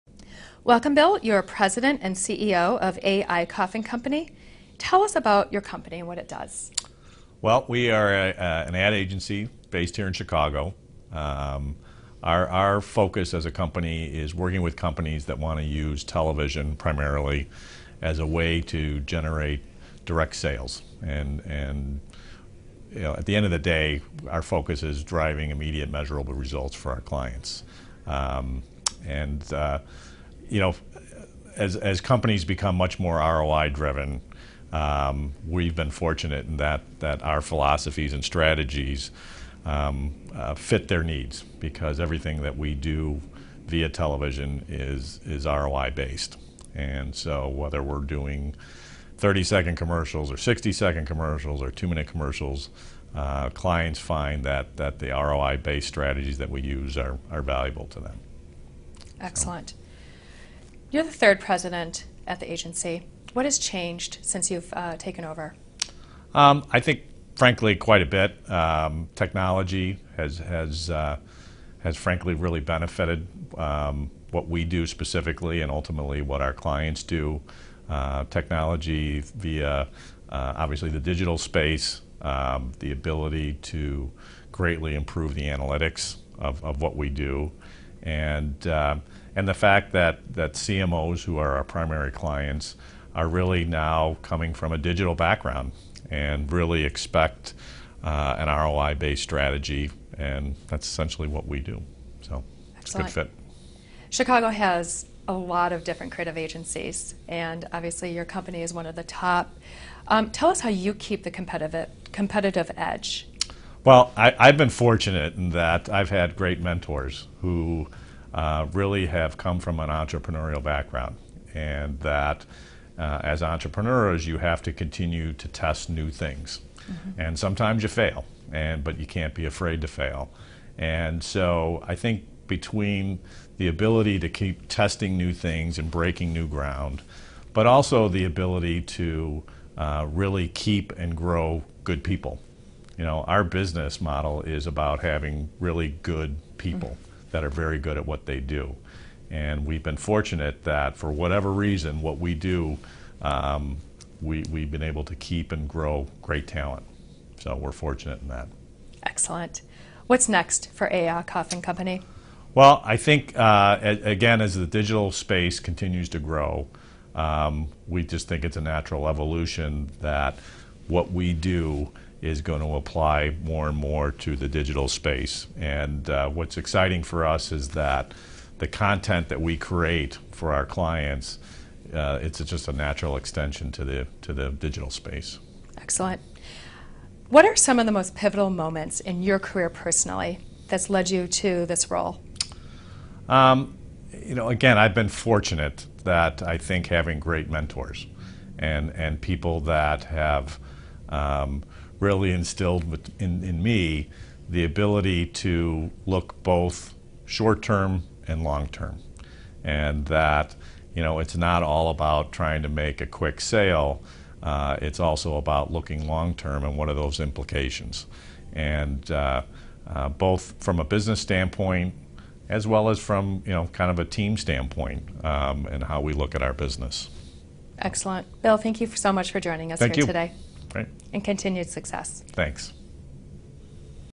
Bill McCabe Interview
CEO of A. Eicoff & Company, an Advertising Agency specializing in DRTV, is interviewed by Ceo Intronet.

Transcript:
Welcome Bill,  you are President and CEO of A. Eicoff & Company. Tell us about your company and what it does.
Well, we are an Ad Agency based here in Chicago. Our focus as a company is working with companies that want to use television primarily as a way to generate direct sales. At the end of the day our focus is driving immediate measurable results for our clients. As companies become much more ROI-driven, we've been fortunate in that our philosophies and strategies fit their needs because everything we do via television is ROI-based. Whether we are doing thirty second commercials or sixty second commercials or two-minute commercials clients find that the ROI-based strategies we use are valuable to them.

Excellent. You are the third president at the Agency. What has changed since you've taken over?
I think frankly quite a bit. Technology has benefited what we do specifically and ultimately what our clients do. Technology via, obviously the digital space, the ability to greatly improve the analytics of what we do, and the fact that CMOs who are our primary clients are really now coming from a digital background and expect and ROI-based strategy. That's essentially what we do, so good fit.

Chicago has a lot of different creative agencies and, obviously, your company is one of the top. Tell us how you keep the competitive edge?
Well, I've been fortunate that I've had great mentors who really have come from an entrepreneurial background. As entrepreneurs you have to continue to test new things and sometimes you fail, but you can't be afraid to fail. I think between the ability to keep testing new things and breaking new ground, but also the ability to keep and grow good people. Our business model is about having really good people that are very good at what they do. We've been fortunate that, for whatever reason, what we do we've been able to keep and grow great talent. So we are fortunate in that.

Excellent. What's next for A. Eicoff & Company?
Well, I think as the digital space continues to grow, we just think it's a natural evolution that what we do is going to apply more and more the digital space. What's exciting for us is that the content we create for our clients naturally extends to the digital space.

What are some of the most pivotal moments in your career personally that's led you to this roll?
Well again, I've been fortunate to have great mentors and people that have really instilled in me the ability to look both short term and long term and that it's not all about trying to make a quick sale, it's also about looking longterm and seeing what those implications are. Both from a business standpoint as well from a team standpoint in how we look at our business.

Bill thank you so much for being with us here today. I wish you continued success.